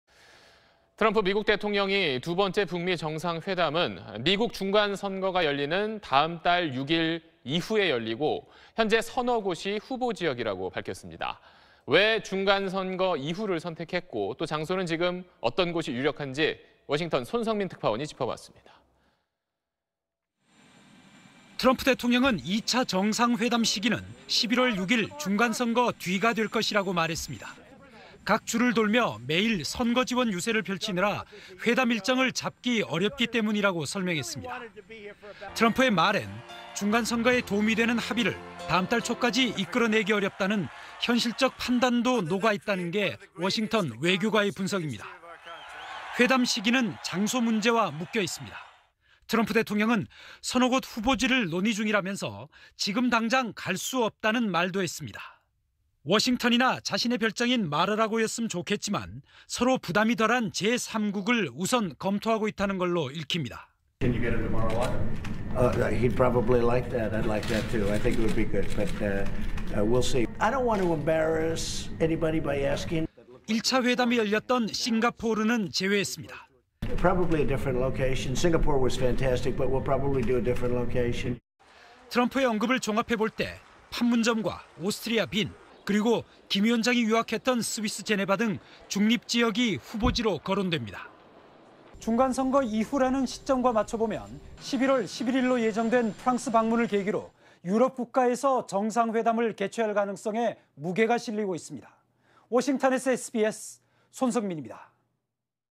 "2차 북미회담 중간선거 이후"…'조기 합의' 기대 접었나 / SBS
트럼프 미국 대통령이 두 번째 북미 정상회담은 미국 중간선거가 열리는 다음 달 6일 이후에 열리고 현재 서너 곳이 후보 지역이라고 밝혔습니다.

왜 중간 선거 이후를 선택했고 또 장소는 지금 어떤 곳이 유력한지 워싱턴 손석민 특파원이 짚어봤습니다.